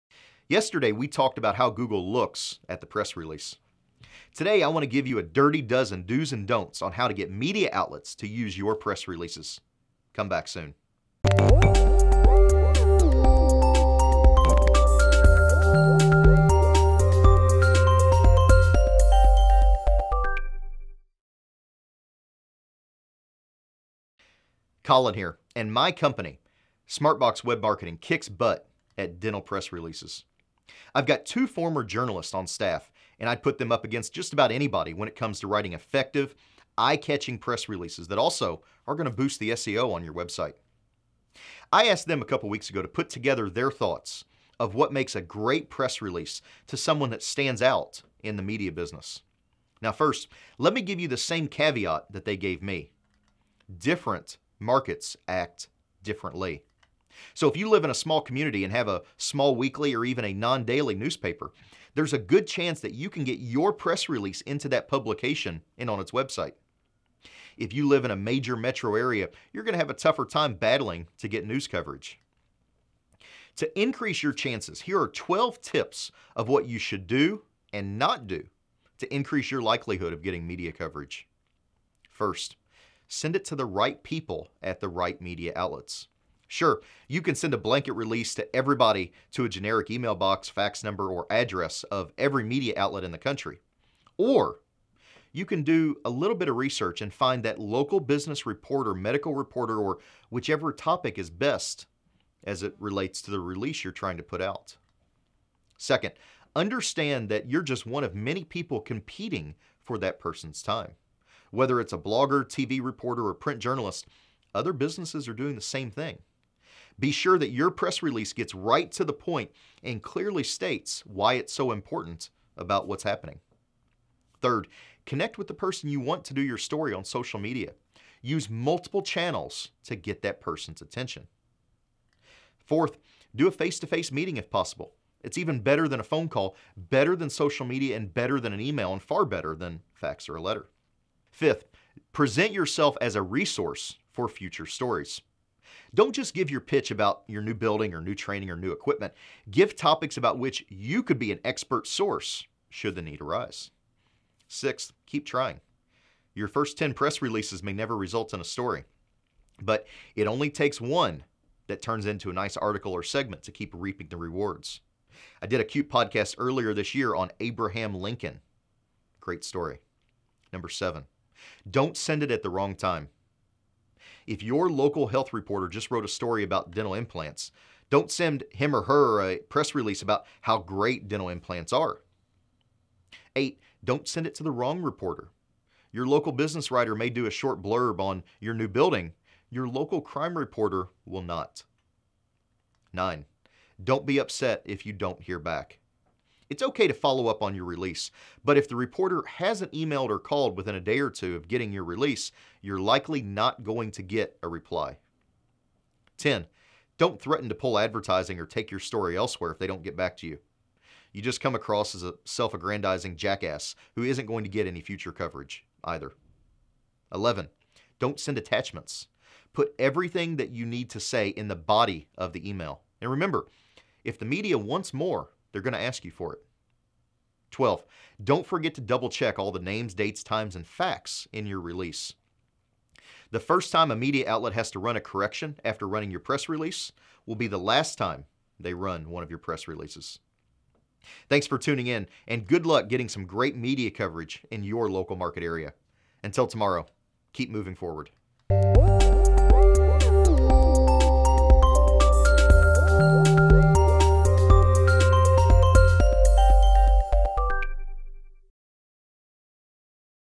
Dirty dozen dental press release tips|SmartBox Web Marketing
Dental press releases that actually get press coverage are a long shot. Colin tells dentists how to beat the odds in Patient Attraction Podcast 447.

For more information on attracting the patients YOU want, go